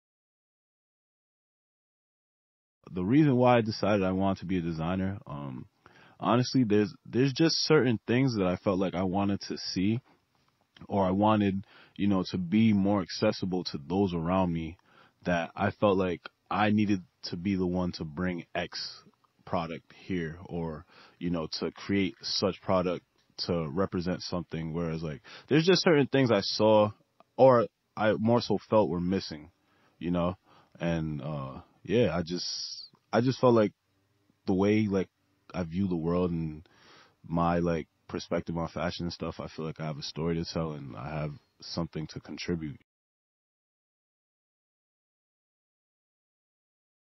A reason to become a designer
Albert Kyei-Poakwa of Pour La Ville @plvofficial on why he became a designer